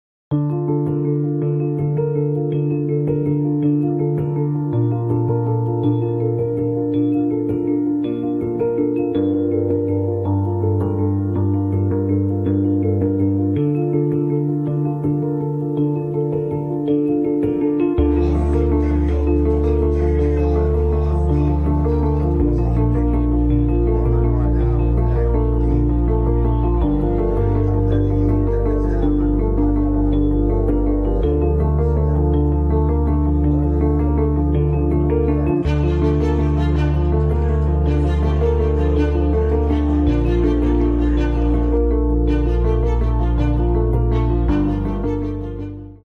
Palestinians take part in a rally marking Islamic Jihad's foundation | WION Shorts
Palestinian Islamic Jihad supporters wave flags as they attend a rally marking the 35th anniversary of the movement's foundation, in Gaza City.